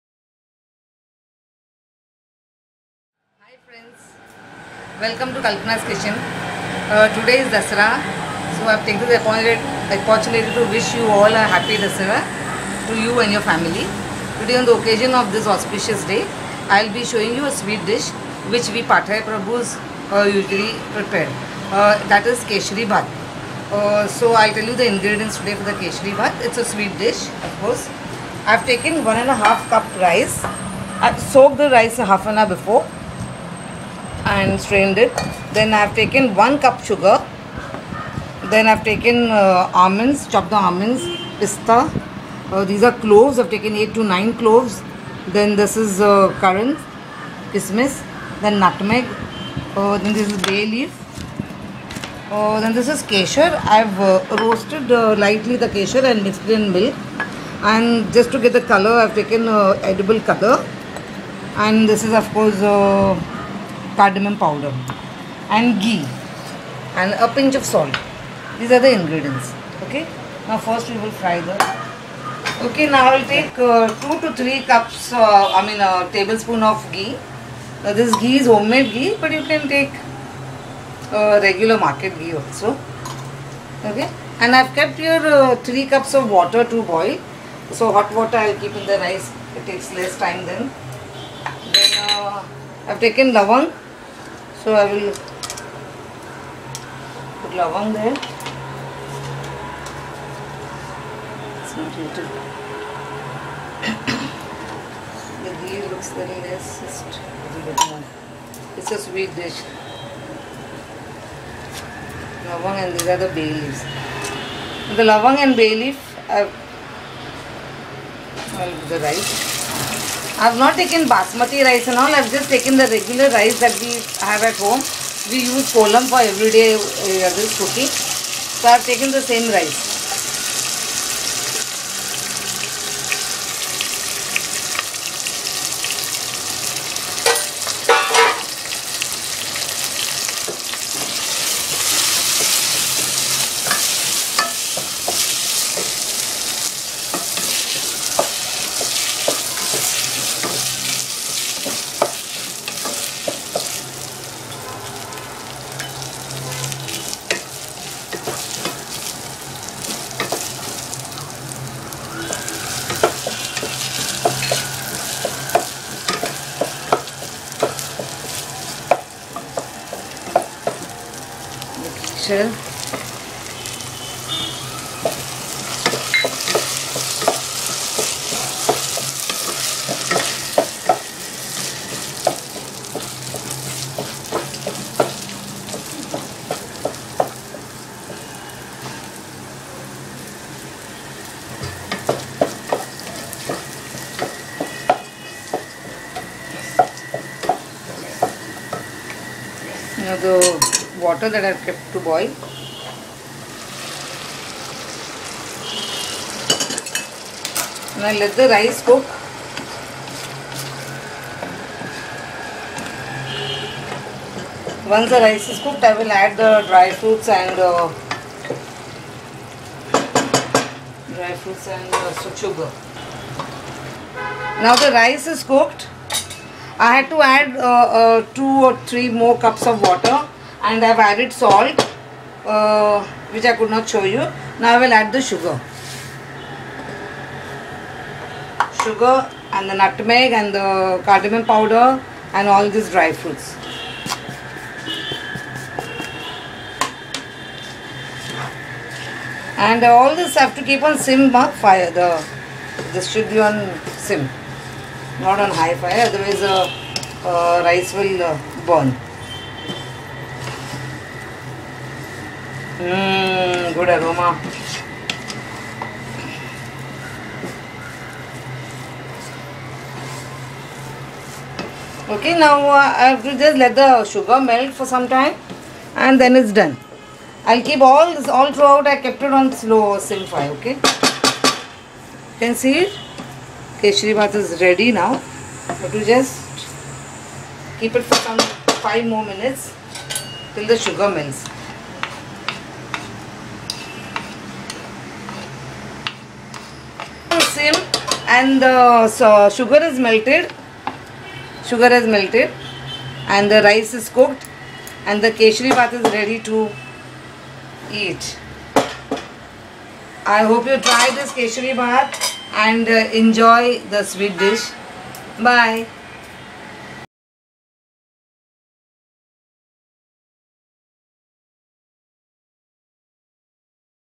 Keshari Bhaat / Kesari Bhat By Kalpana Talpade / केशरी भात /Saffron Rice
Sweet saffron rice | Keshri Bhaat By Kalpana Talpade

Sweet Saffron Rice prepared in Pathare Prabhu style.  This sweet dish is prepared during Dasera and is accompanied with coconut milk curry called tomato chi sheer.